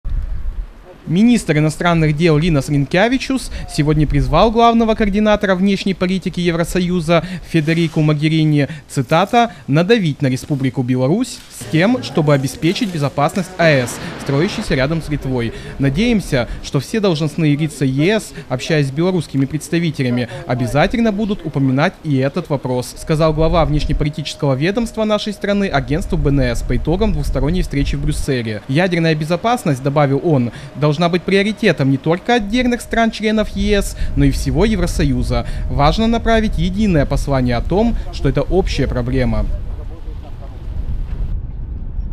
Беларусь – главный оппонент в энергетике
Совсем другие настроения в энергетическом сотрудничестве Литвы и Беларуси. Сегодня глава МИД Литвы призвал Еврокомиссию оказать давление на официальный Минск в вопросе строительства БелАЭС на границе с Литвой.